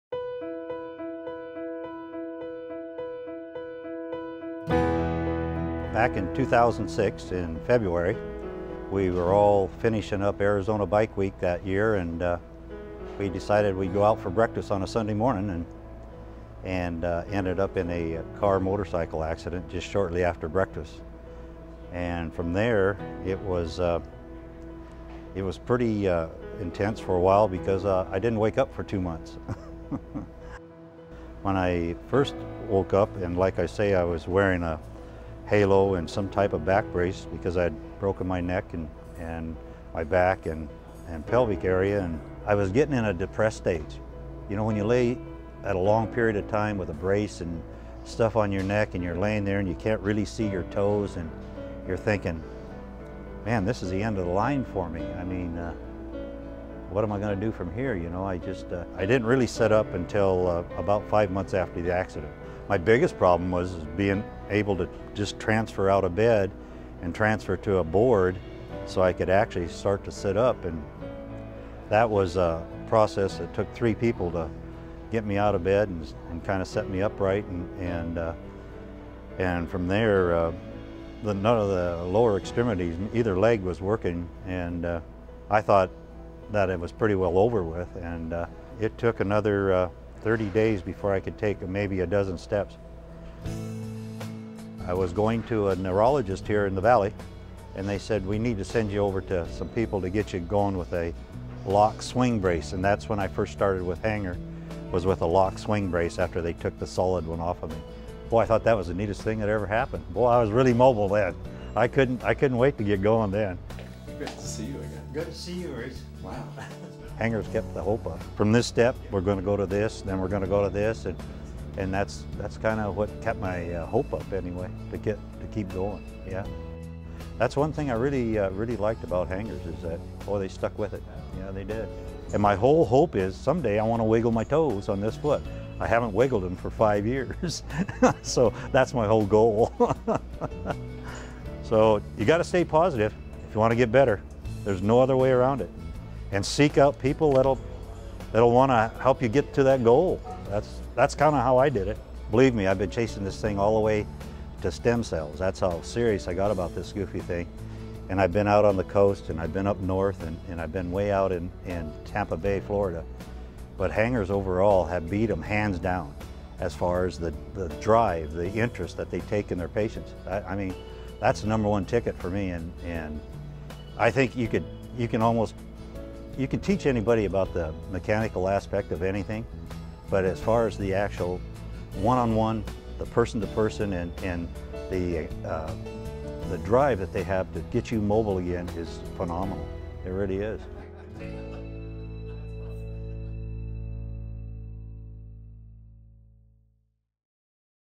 Anson Saccomanno Profile - Hanger Clinic
Anson's neck was broken when he was involved in a car / motorcycle accident. Using orthotics, he now has a new level of mobility.